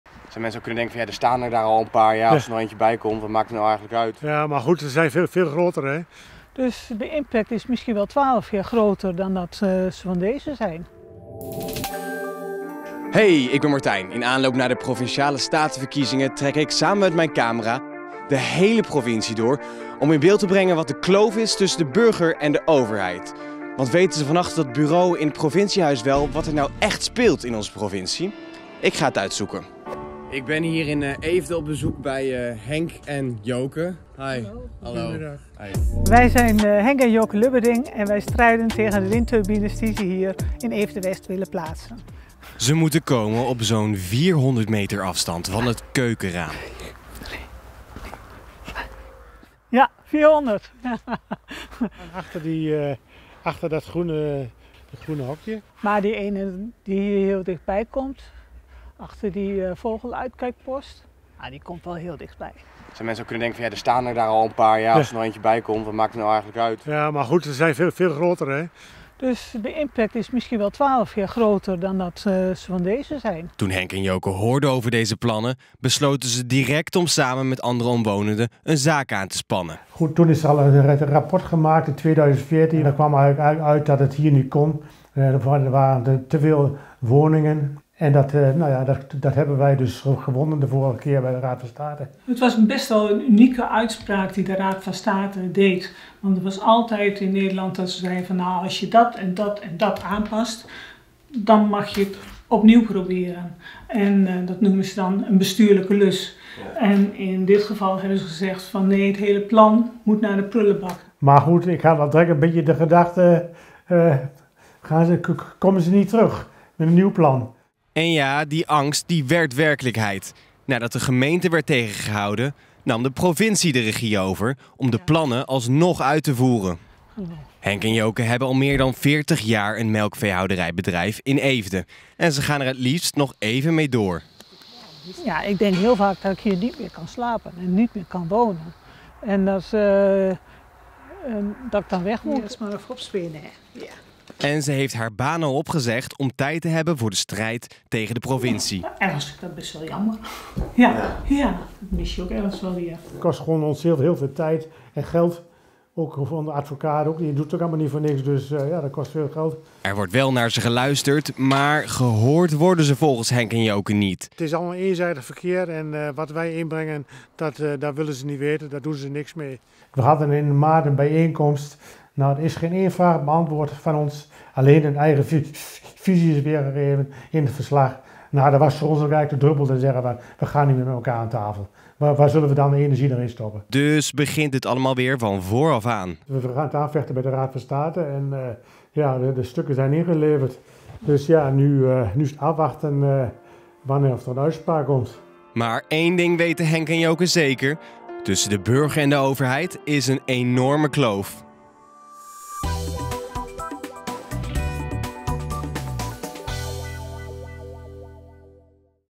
Henk en Joke wonnen rechtzaak tegen komst windmolens maar ze werden toch gebouwd
Het zal je gebeuren: je dient bezwaar in tegen de komst van windmolens, krijgt gelijk bij de rechter, maar vervolgens komen de windmolens er tóch. Het is het verhaal van Henk en Joke Lubberding uit Eefde. Al het nieuws uit Gelderland volgen?